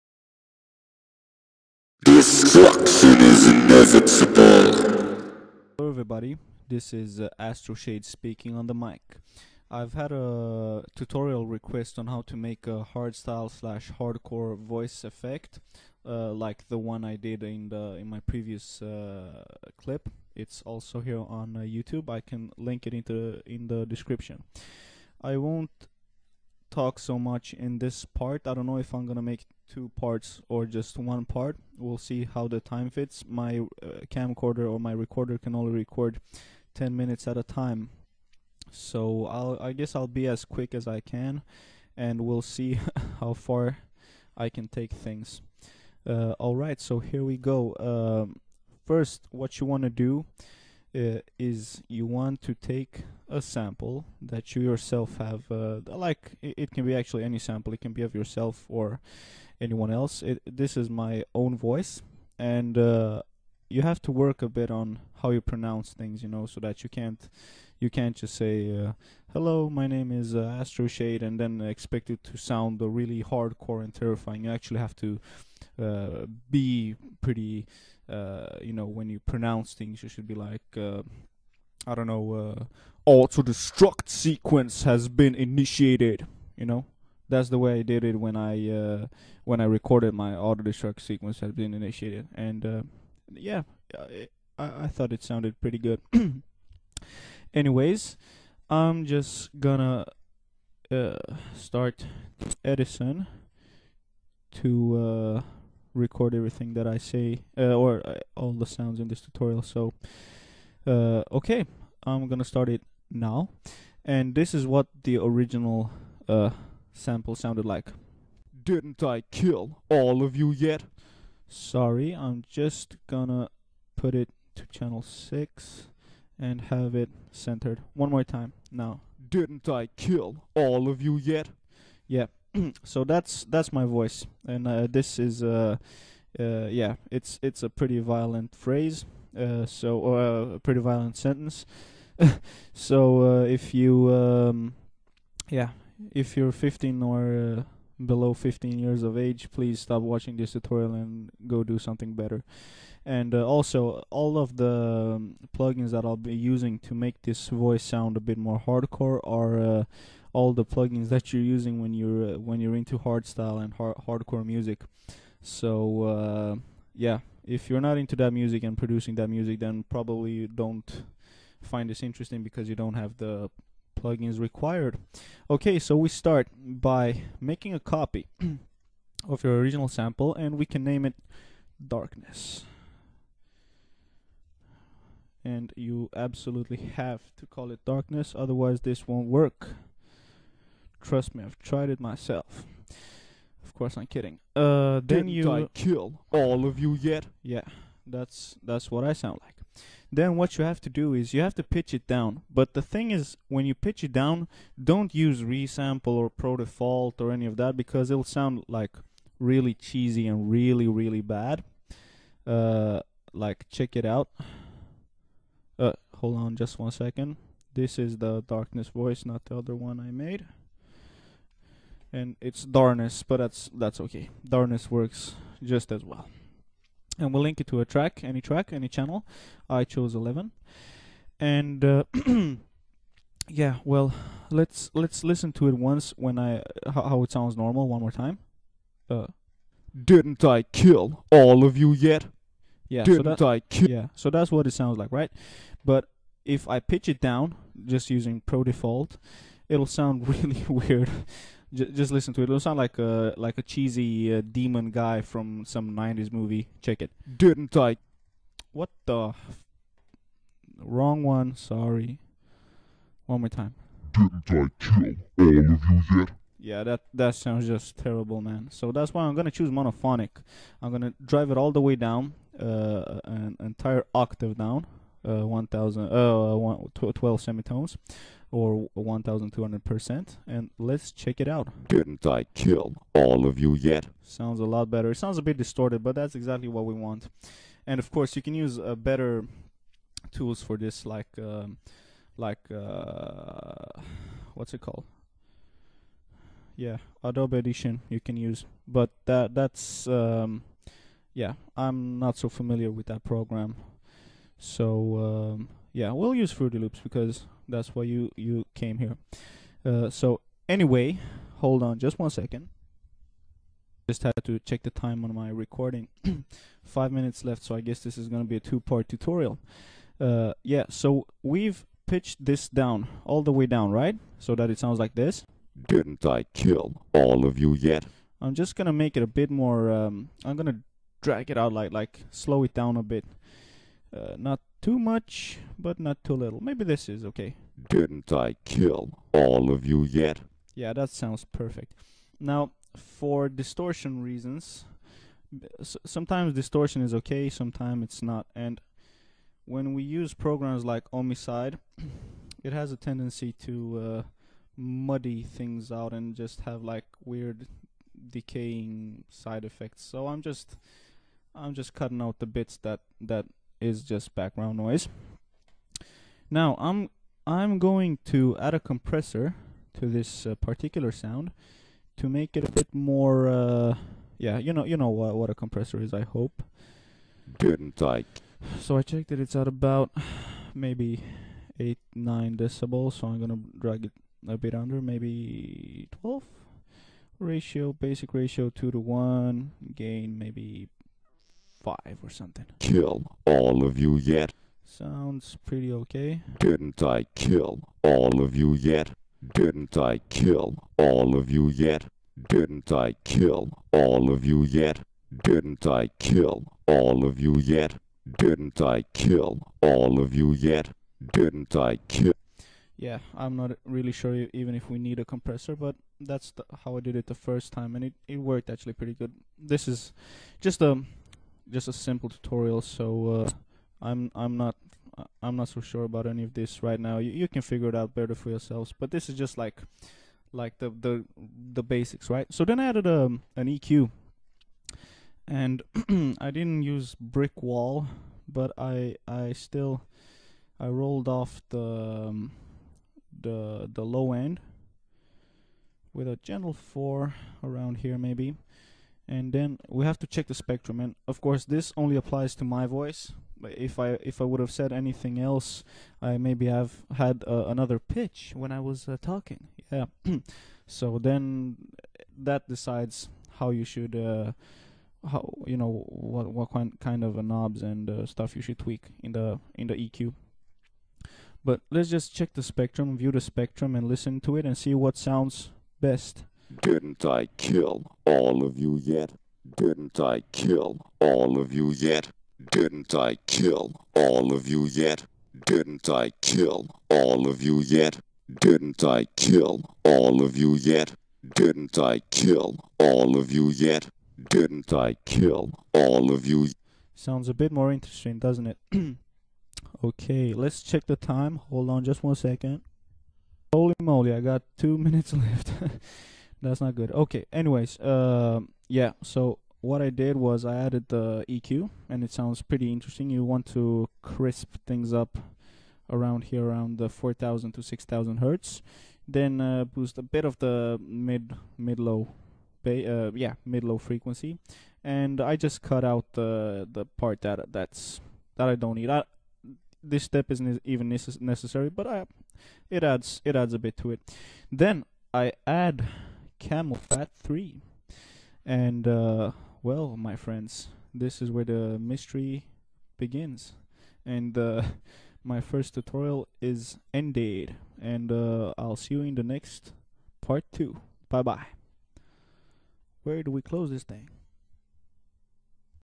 Hardstyle / Hardcore Voice Effect FL Tutorial [by Astroshade] Part 1 of 2
FIRTS OFF!... Sorry for the panned mic and low volume... I have a new mic and I'm completely new to recording tutorials and then making movies...

This is my first video (and audio)-tutorial ever :)

VSTs used:

- Fruity loops Parametric EQ 2
- Fruity Compressor
- Camelphat 3
- Ohmicide
- Karma FX reverb

If ANYTHING is unclear or you have questions reguarding this video or similar effects post a comment on this video or the next and I'll do my best to give you a good answer :)